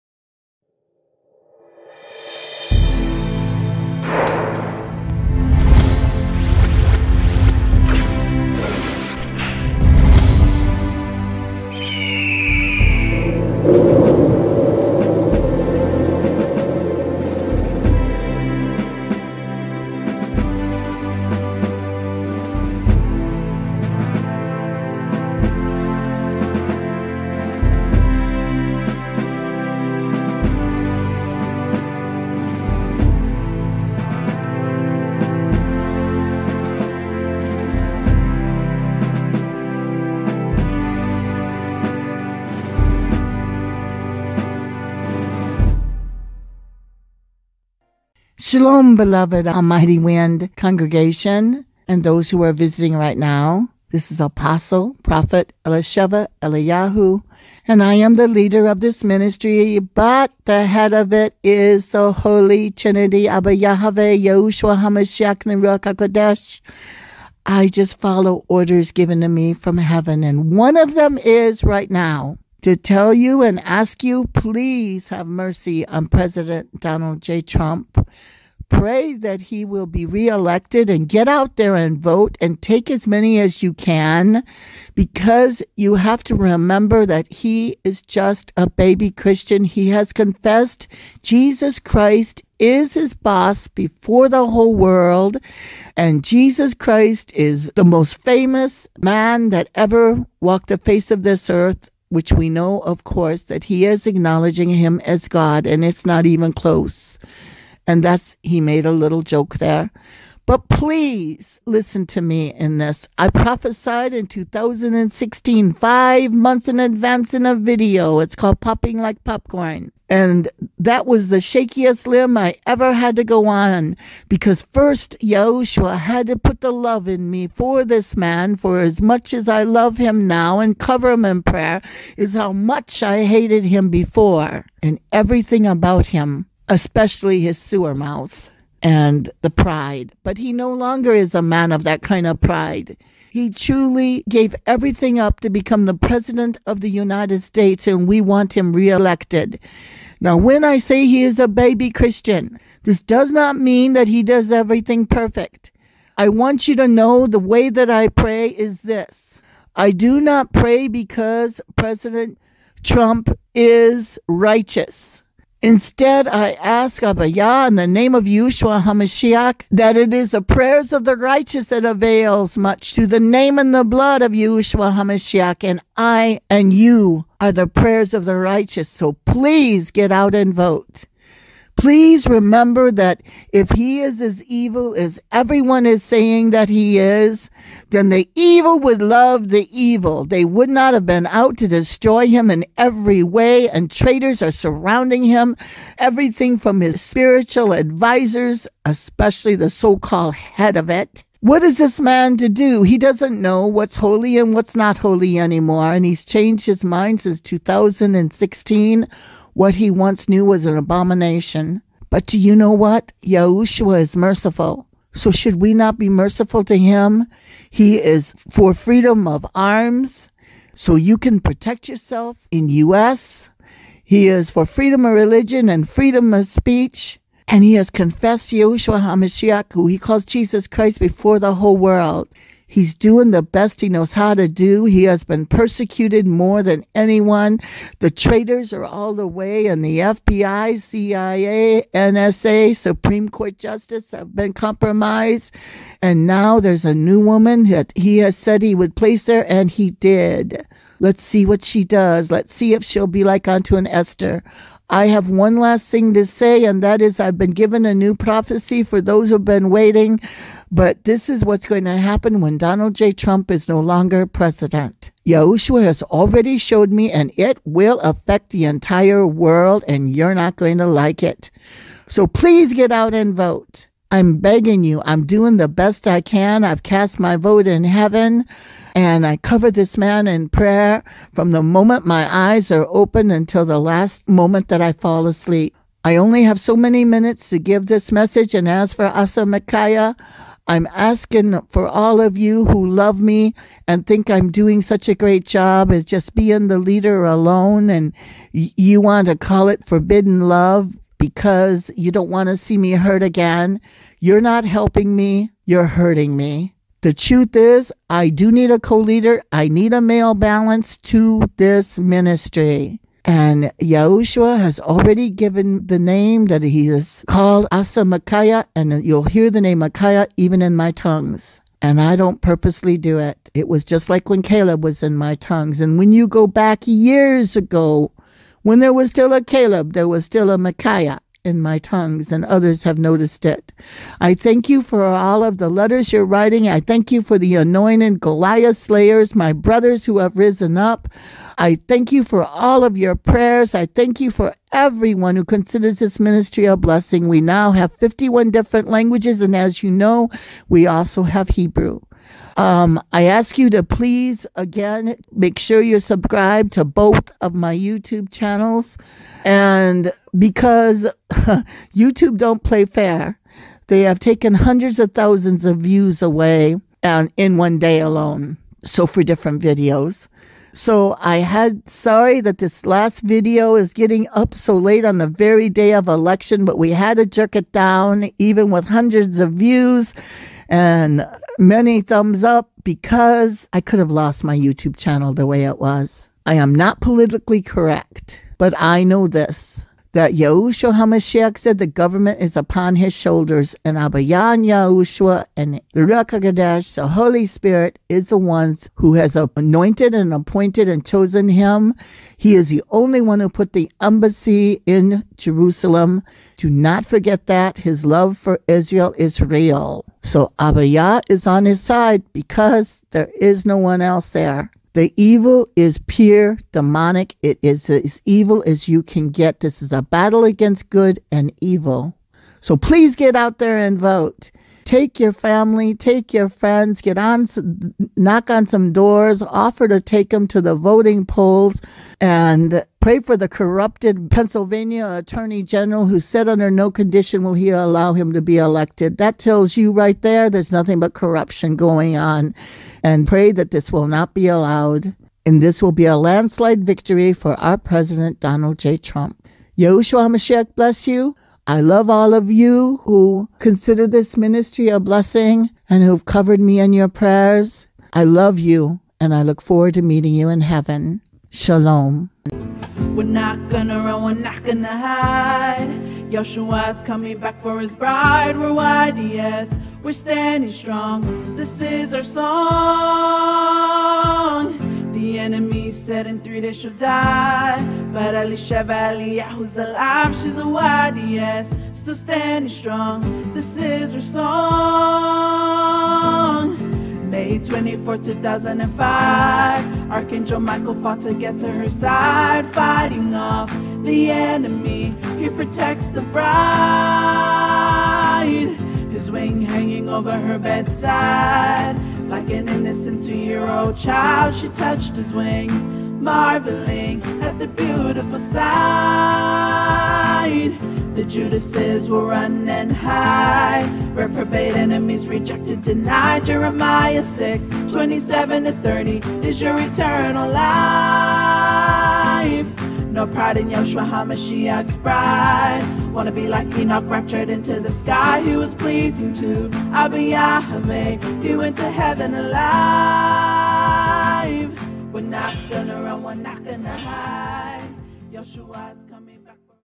Everything Hidden In The Darkness Will Be Revealed In The Light
Mirrored from YAHSladyinred
Originally published on Nov 6, 2020

Please join us in prayer as we intercede for President Donald J Trump and for the ElectionResults to be fair and for him to once again win this Presidential election. In 2016 I prophesied to the world 5 months prior to him winning the Presidential election in 2016. We pray for high and uncommon favor for Donald J Trump especially during this time where there is alot of corruption and dishonesty in the counting of the ballots and we pray that all those who voted for Trump will be counted for. YAHs will be done in YAHUSHUA HA MASHIACH, Amen.